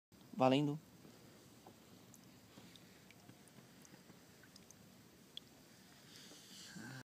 Robalo Peva (HD)
Robalo peva pego em rio de água "coca cola" no camarão D.O.A